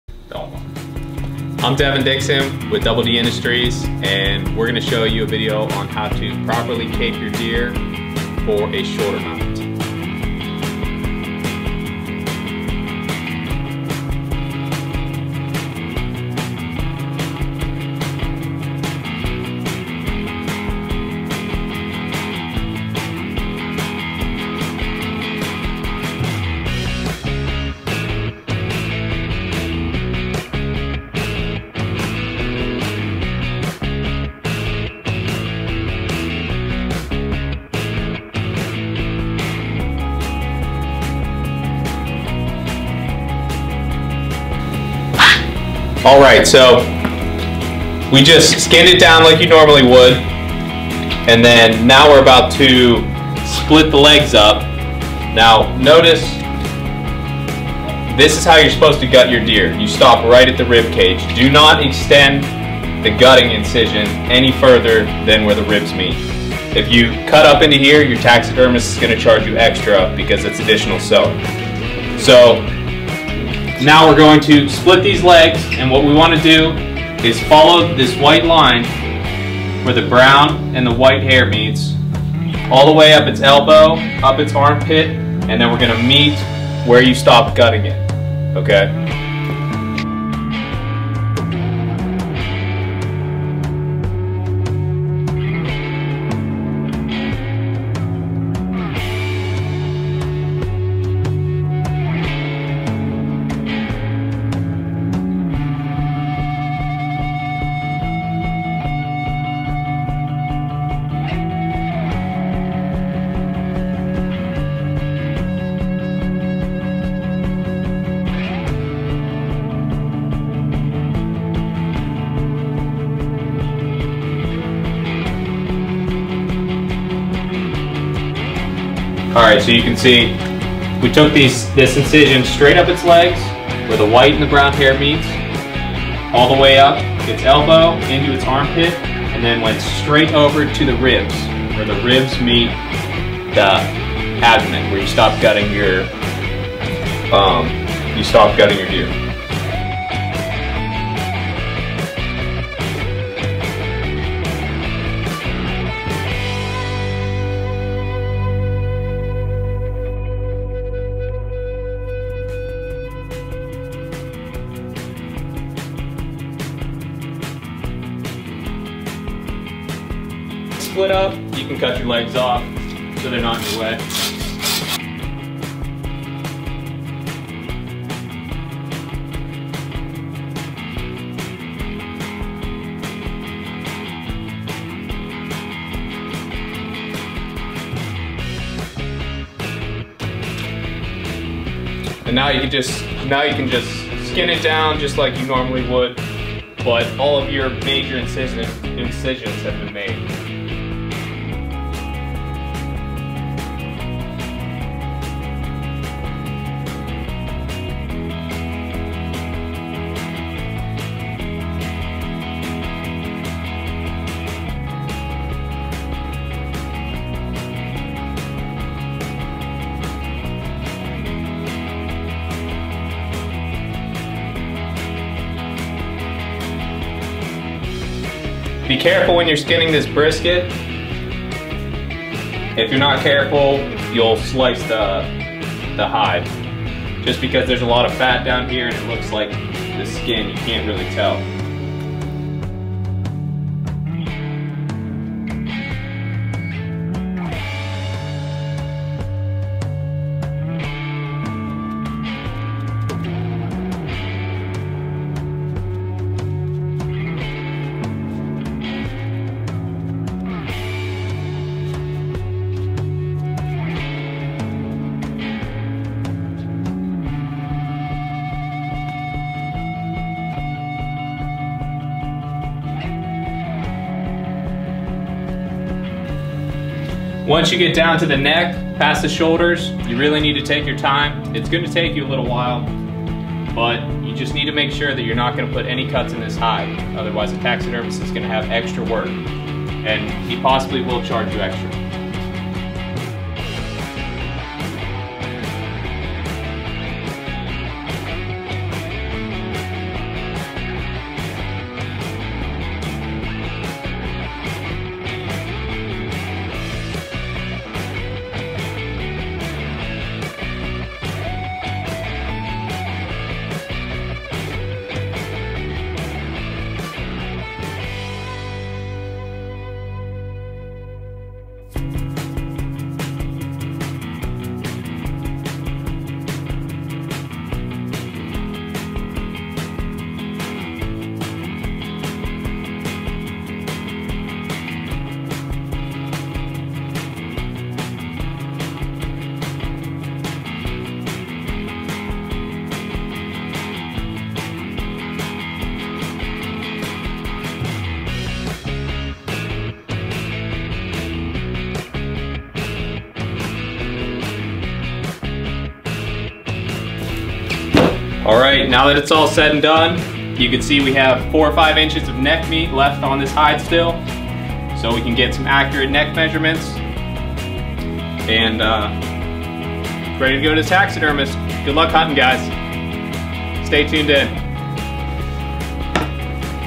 How to properly cape a deer for mounting
Devan Dickson of DD Industries shows us how to properly cape a buck to head to the taxidermist